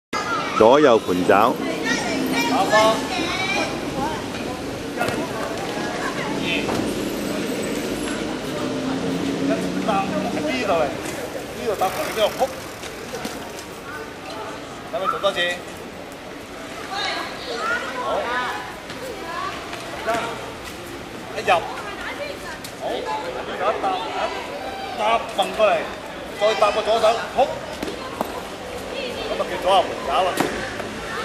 rope dart - left and right elbow
performed by Master Ng Kwok Cheung